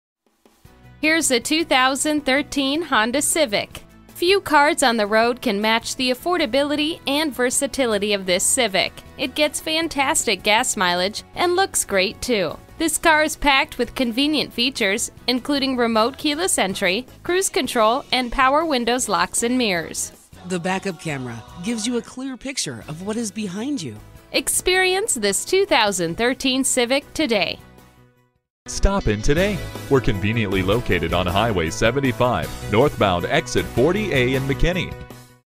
2013 Honda Civic Sdn Dallas TX McKinney, TX
Year: 2013
Make: Honda
Model: Civic Sdn
Trim: 4dr Auto LX Sedan
Engine: 1.8L SOHC MPFI 16-VALVE I
Transmission: 5-Speed Automatic
Color: Taffeta White
Interior: Beige
Stock #: DH599906
This 2013 Honda Civic Sdn 4dr 4dr Auto LX Sedan features a 1.8L SOHC MPFI 16-VALVE I 4cyl Gasoline engine. It is equipped with a 5 Speed Automatic transmission. The vehicle is Taffeta White with a Beige interior. It is offered with a full factory warranty. - Air Conditioning, Cruise Control, Power Windows, Power Mirrors, Phone, Clock, Tachometer, Tilt Steering Wheel, Steering Wheel Radio Controls, Side Airbags, Security System, ABS Brakes, Traction Control, Dynamic Stability, Rear Defogger, Intermittent Wipers, Alloy Wheels, 1.8L SOHC MPFI 16-valve i-VTEC I4, Fold-down rear seatback -inc: adjustable head restraints, Center console w-sliding armrest, Front & rear floor mats, Driver footrest, Indicator lights -inc: trunk-door-open, Intelligent multi-information display (i-MID) -inc: 5 LCD screen, Headlights-on reminder, Maintenance Minder system, Pwr door locks w-auto-lock feature, Illuminated driver window-driver door lock controls, Remote entry -inc: remote trunk release, Remote fuel filler door release, Interior remote trunk release w-lock, Rear seat heater ducts, Front 12V aux pwr outlet, Coin tray, Front cup holders, Front door pocket storage bins, Dual visor vanity mirrors, Map lights, Cargo area light, P195-65R15 all-season tires, Body-colored bumpers, Multi-reflector auto-off halogen headlights, Body-colored door handles, Drive-by-wire throttle, Eco Assist system, MacPherson strut front suspension, Multi-link rear suspension, Front & rear stabilizer bars, Motion-adaptive electric pwr rack & pinion steering, Brake assist, Advanced Compatibility Engineering (ACE) II bod

Address:
601 S Central Expy